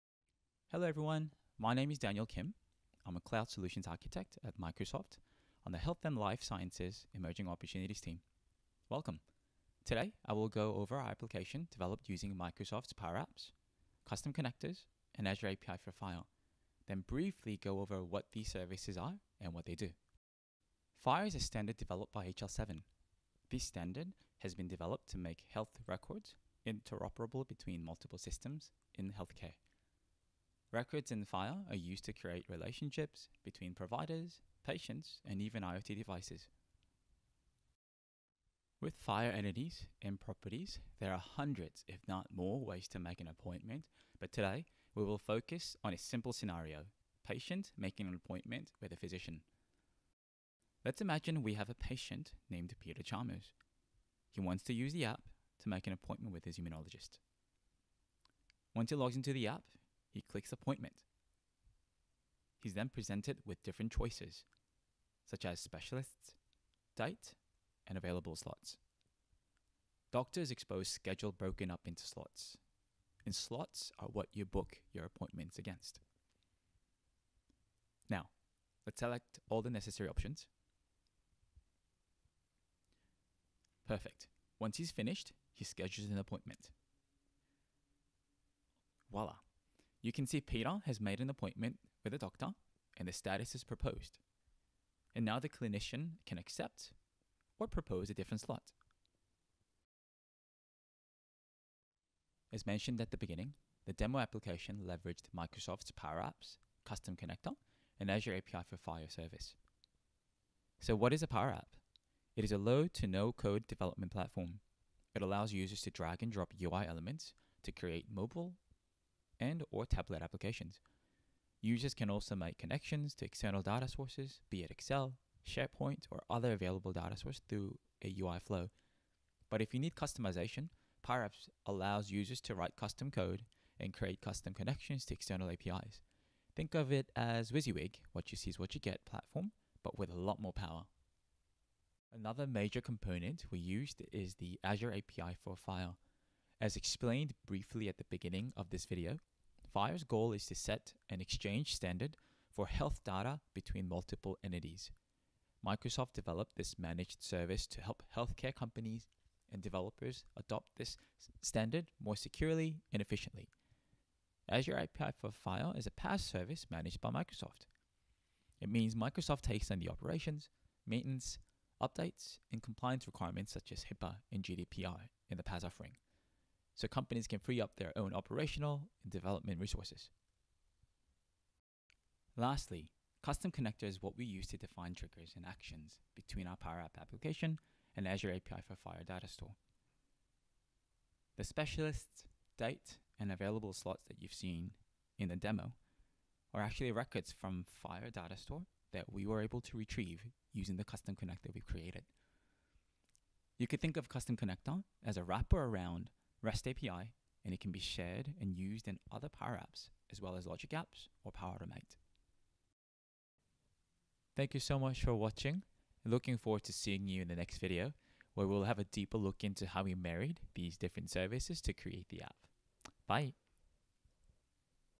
FHIRPower Introduction
We show you how easy it is to make a scheduling app between a Patient and a doctor with PowerApps and Azure API for FHIR!

This video is a brief introduction to the app and shows you what technology went in to develop it.

(We will post another video which will go deeper into PowerApps Component Framework and how we leveraged it in our app)